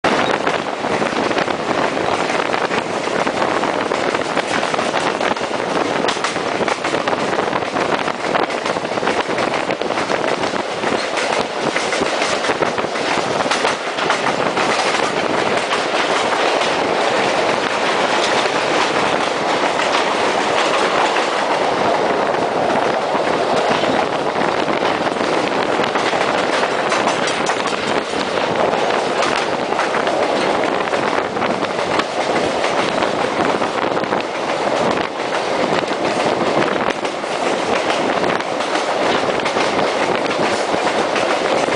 Riding Behind the NKP 765 at Carland,MI on 7/25/09
This video was taken on board the train riding behind the Nickel Plate Road (NKP) 765 on July 25,2009 at Carland,Michigan.